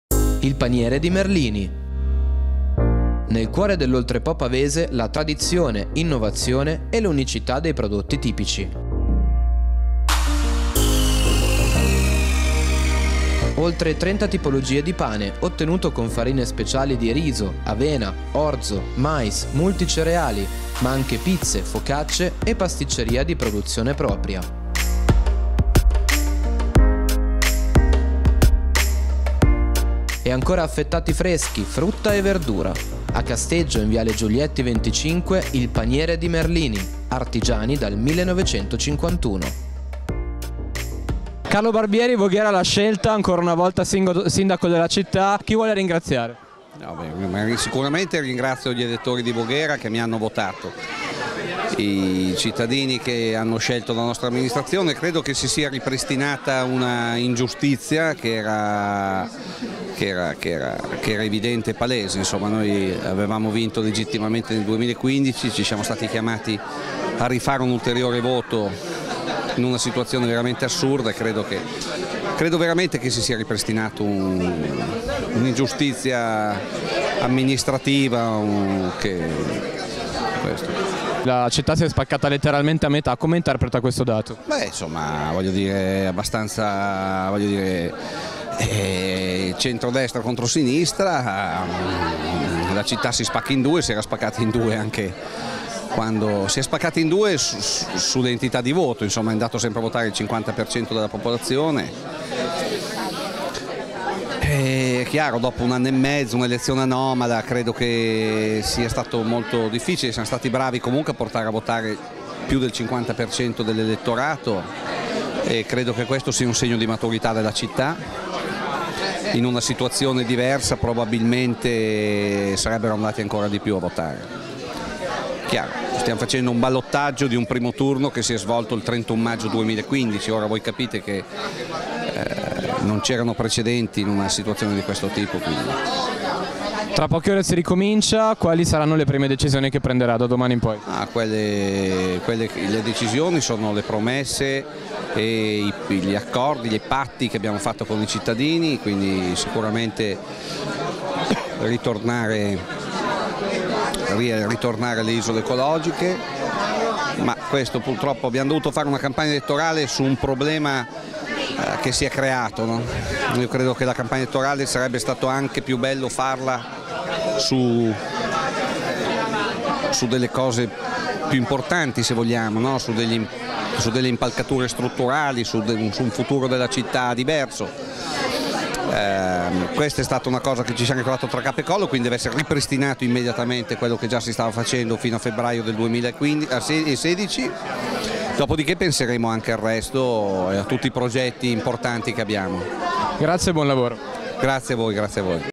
Intervista a Barbieri dopo la vittoria: “si è ripristinata un’ingiustizia palese”
Produzione: Jab Media